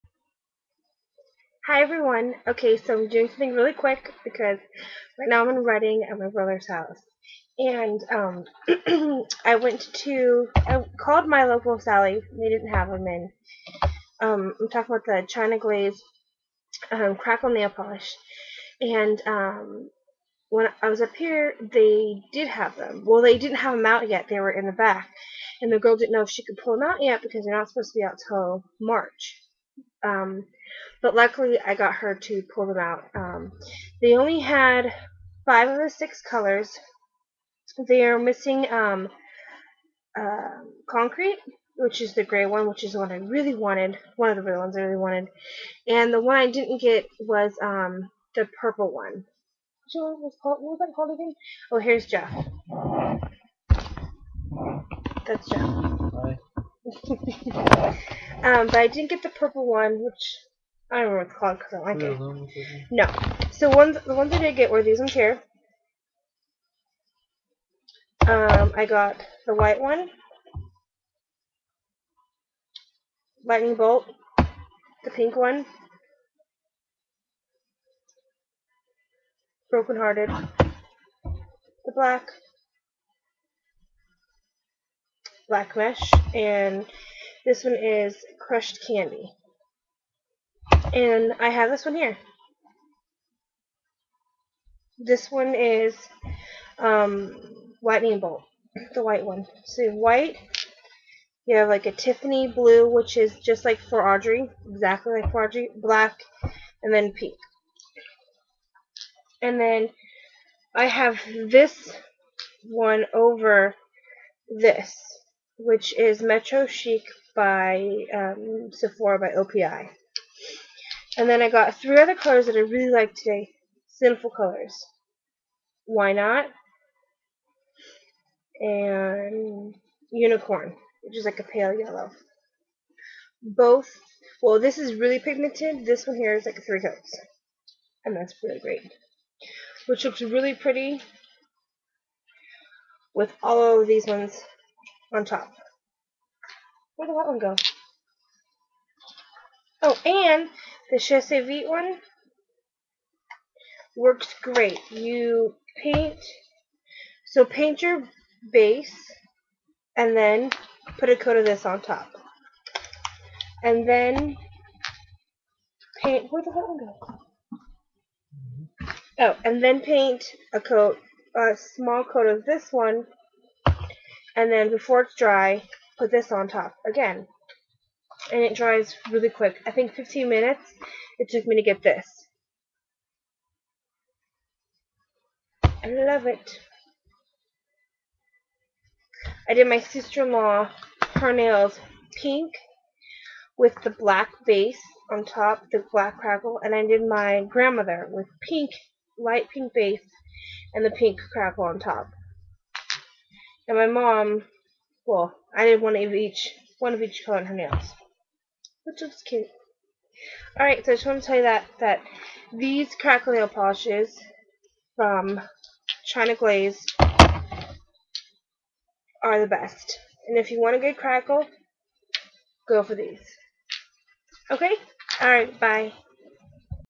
China Glaze Crackle
The new Crackle Glaze from China Glaze. They have 6 new pigmented colors for this line.
1. Lightning Bolt / White
2.Broken Hearted / Pink
3.Black Mesh / Black
4.Crushed Candy / same as For Audrey blue
5.Crackled Concrete / Gray
6.Fault Line / Purple
I have 1-4 and going to get Crackled Concrete, however i have heard and see that Fault line just doesn't cut the cake so no need to waste my money. for a fast and fun nail look you should give it a try.